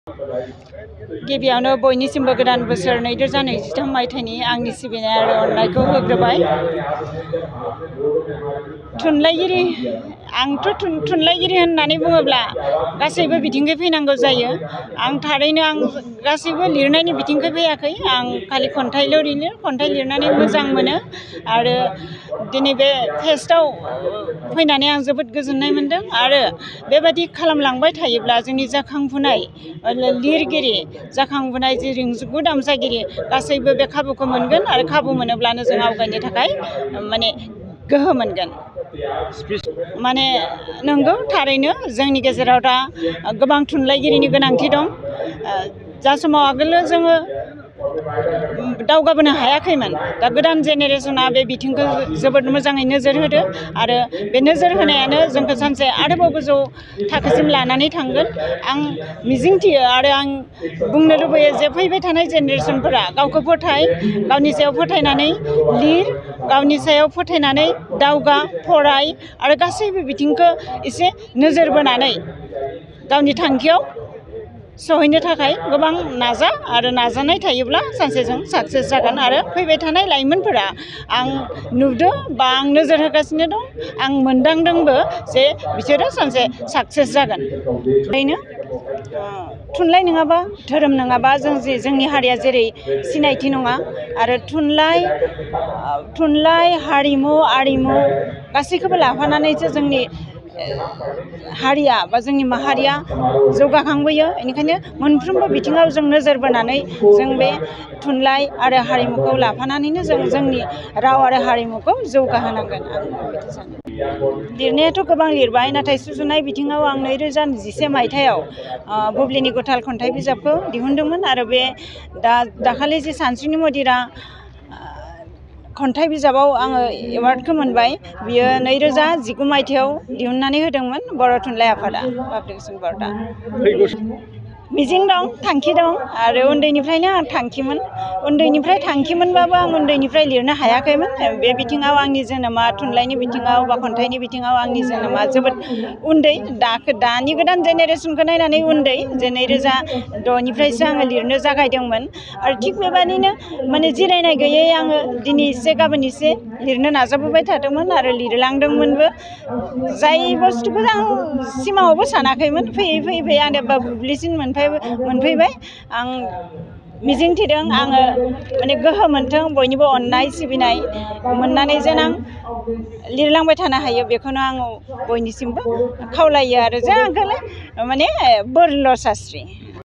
"सानस्रिनि मदिरा" खन्थाइ बिजाबनि थाखाय साहित्य अकाडेमि बान्था 2022 मोननो हानाय रस्मि सौधुरीआ कक्राझ
"सानस्रिनि मदिरा" खन्थाइ बिजाबनि थाखाय साहित्य अकाडेमि बान्था 2022 मोननो हानाय रस्मि सौधुरीआ कक्राझार थुनलाइआरि फोर्बोआव बाहागो लाफैना मा फोरमायखो